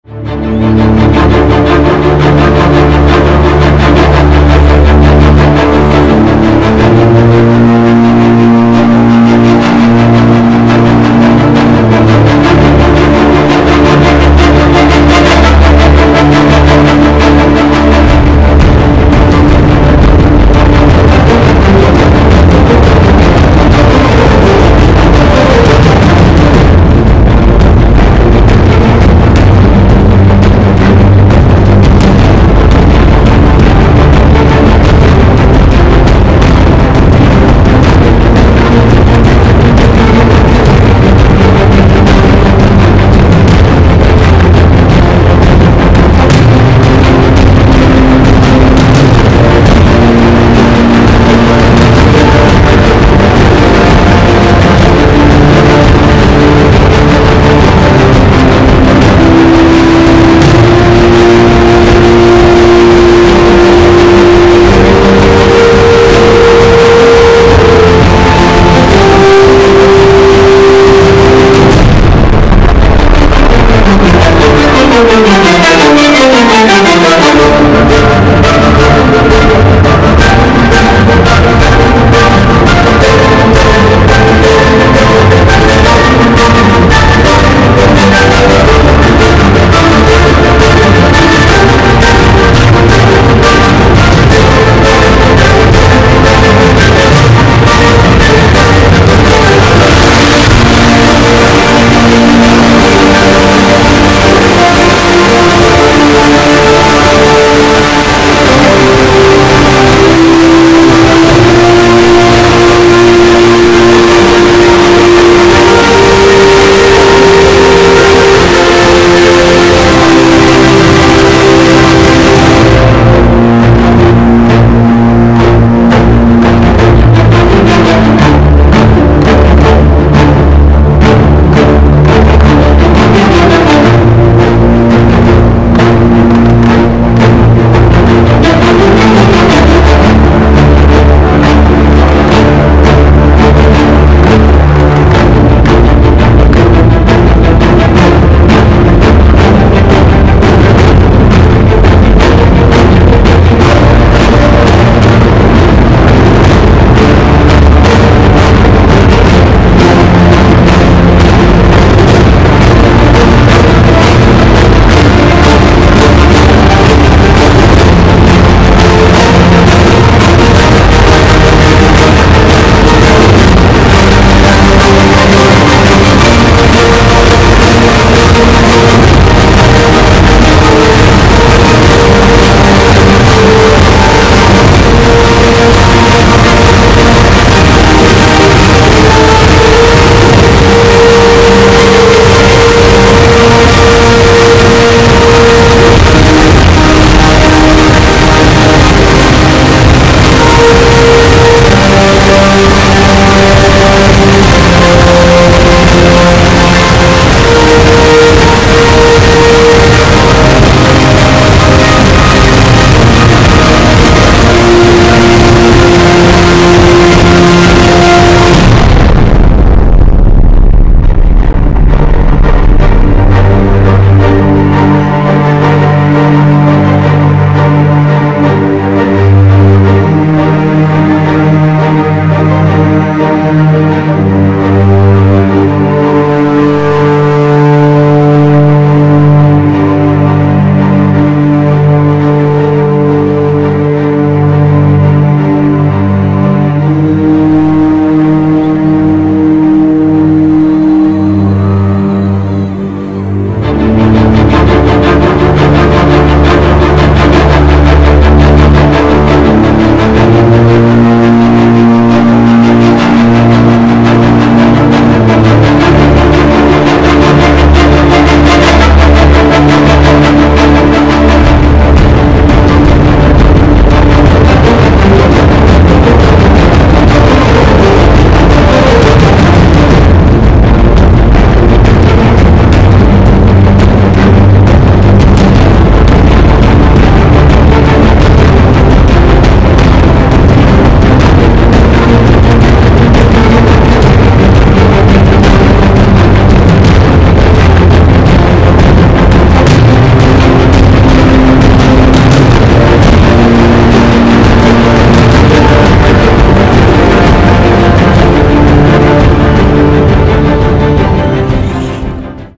Axial SCX10 2. Competition. Last part.
Третий Спец участок в Калуге:
Third special stage in Kaluga city:
🔻Axial  SCX10-II Marlboro
▶︎ Castle Creations Sidewinder 4
▶︎ Tenshock 906 2400 kw Motor
▶︎ LiPo 3S 11.1V
▶︎ Beadlock 1.9 Wheels
▶︎ Gmade 1.9 MT 1904 Off-road Tires (GM70304)